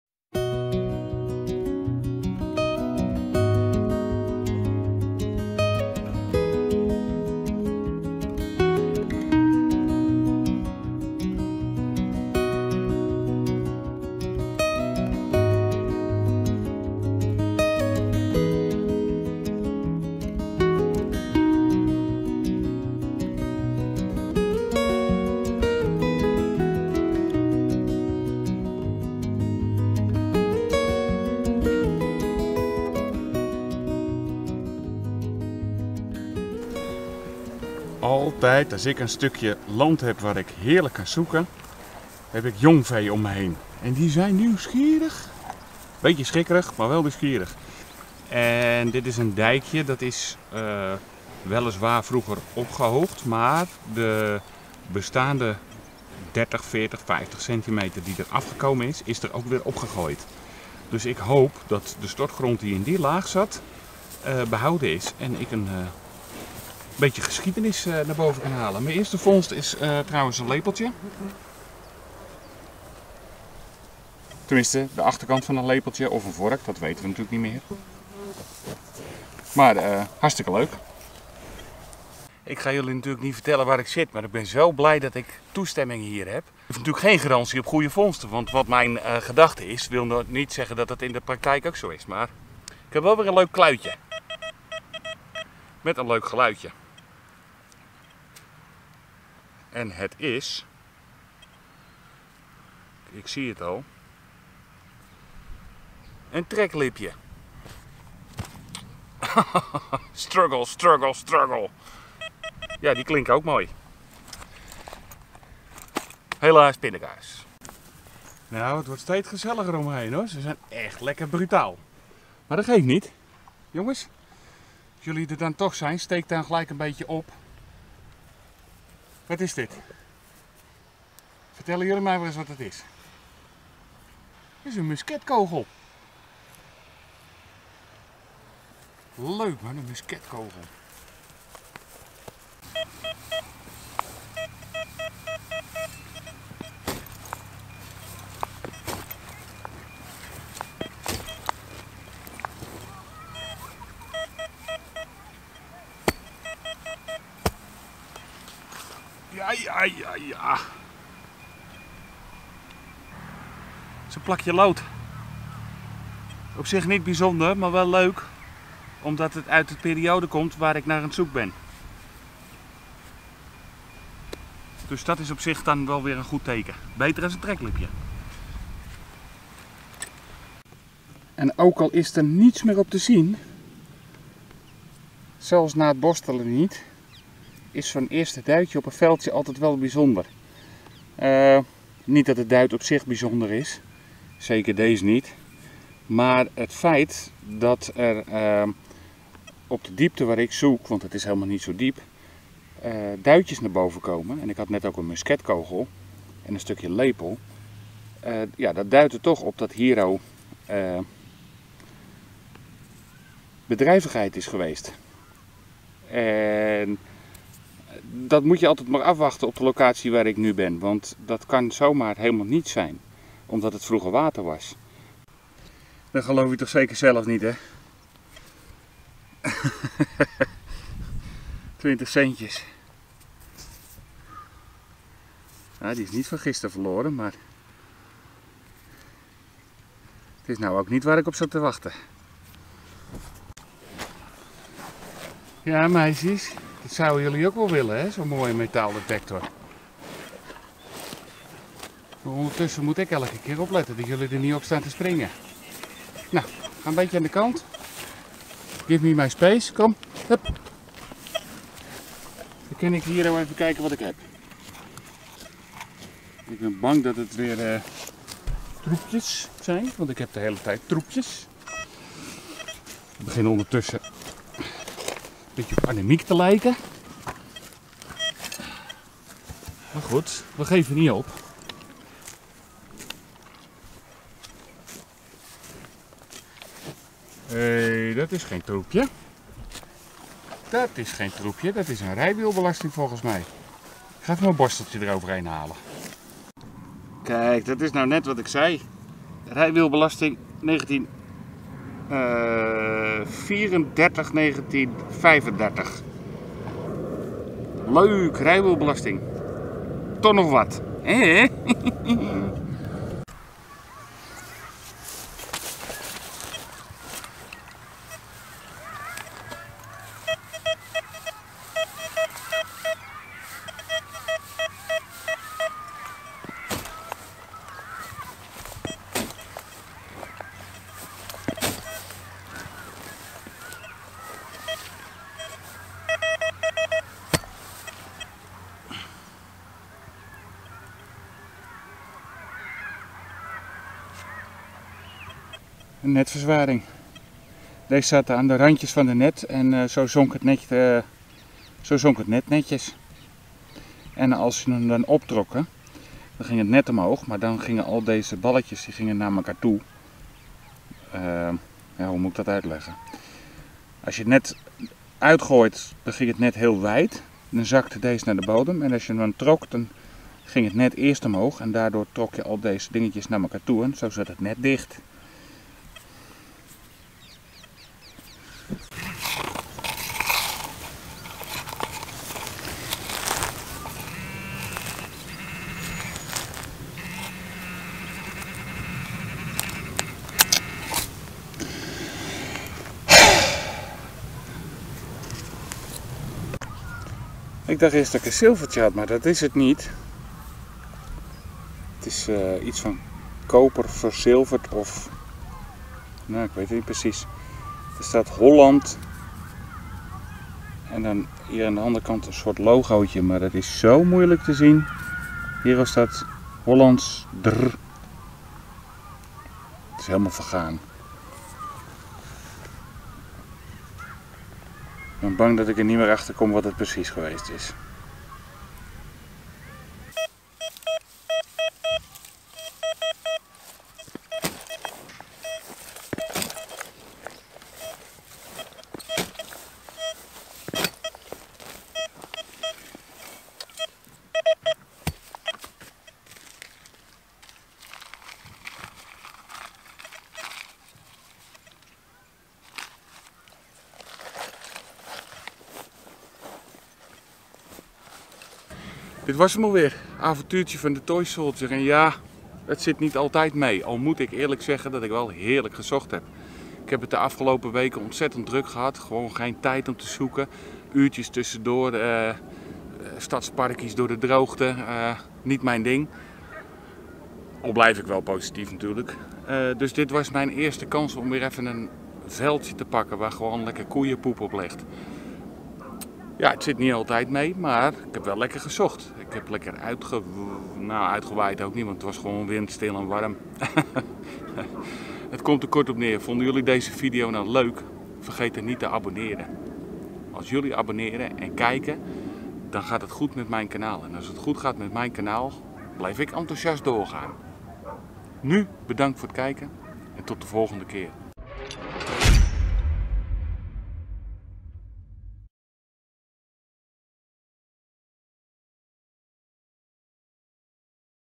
met de metaaldetector tussen de meiden zoeken metaaldetectie
met de metaaldetector tussen de koeien zoekend naar oude bodemvondsten ...
deze manier van metaaldetectie vind ik het leukst ...